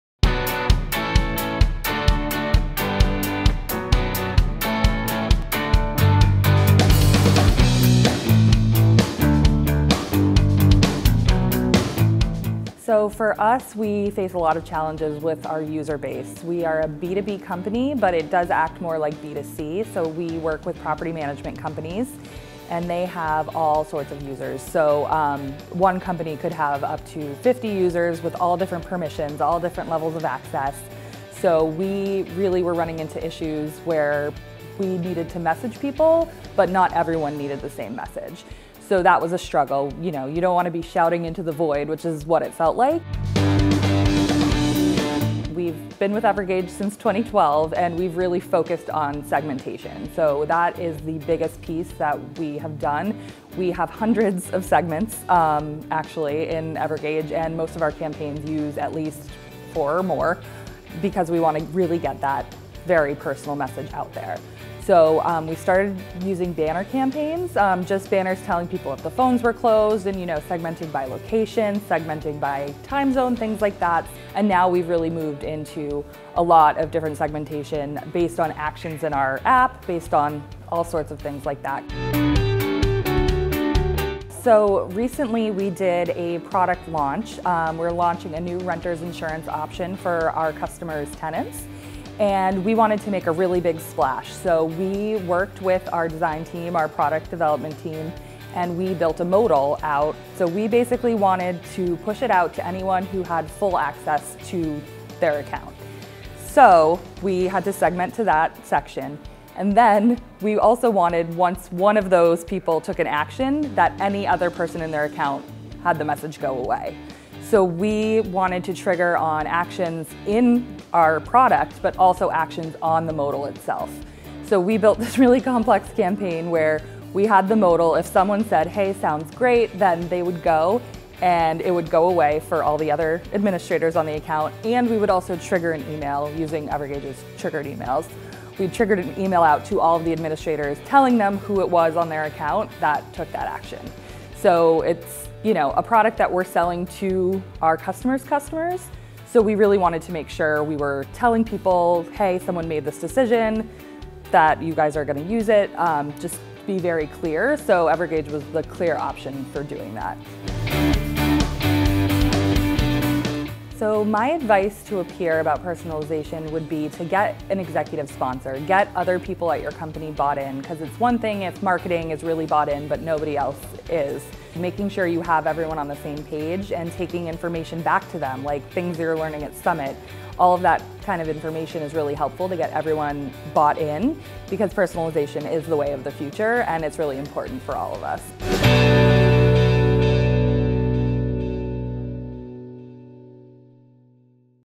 Buildium Customer Success Story
Elyse Leblanc-Gerakines from Buildium describes how delivering in-app messages and triggered emails to their SaaS platform users improves customer experiences. To learn more about how personalization can help you improve your customers’ experiences,

For us, we face a lot of challenges with our user base. We are a B2B company, but it does act more like B2C. We work with property management companies, and they have all sorts of users. One company could have up to 50 users, with all different permissions, all different levels of access. So, we really were running into issues where we needed to message people, but not everyone needed the same message. So, that was a struggle. You don’t want to be shouting into the void, which was what it felt like.

We’ve been with Evergage since 2012, and we’ve really focused on segmentation. That is the biggest piece that we have done. We have hundreds of segments, actually, in Evergage, and most of our campaigns use at least four or more, because we want to really get that very personal message out there. So, we started using banner campaigns; just banners telling people that the phones were closed, segmenting by location, segmenting by time zone, things like that. And now, we’ve really moved into a lot of different segmentation based on actions in our app, based on all sorts of things like that.

Recently we did a product launch. We were launching a new renter’s insurance option for our customers’ tenants, and we wanted to make a really big splash. So, we worked with our design team, our product development team, and we built a modal out. We basically wanted to push it out to anyone who had full access to their account. So, we had to segment to that section, and then we also wanted – once one of those people took action – that any other person in their account had that message go away. We wanted to trigger on actions in our product, but also actions on the modal itself.

So, we built this really complex campaign where we had the modal if someone said, “Hey, sounds great,” then they would go and it would go away for all the other administrators on the account, and we would also trigger an email using Evergage’s triggered emails. We triggered an email out to all the administrators, telling them who it was on their account that took that action. It’s a product that we’re selling to our customers’ customers, so we really wanted to make sure we were telling people, “Hey, someone made this decision that you guys are going to use it.” Just be very clear. So, Evergage was the clear option for doing that.

My advice to a peer about personalization would be to get an executive sponsor, get other people at your company bought in. Because it’s one thing if marketing has really bought in but nobody else is. Making sure you have everyone on the same page, and taking information back to them, like things you’re learning at summit, all of that kind of information is really helpful to get everyone bought in. Because personalization is the way of the future, and it’s really important for all of us.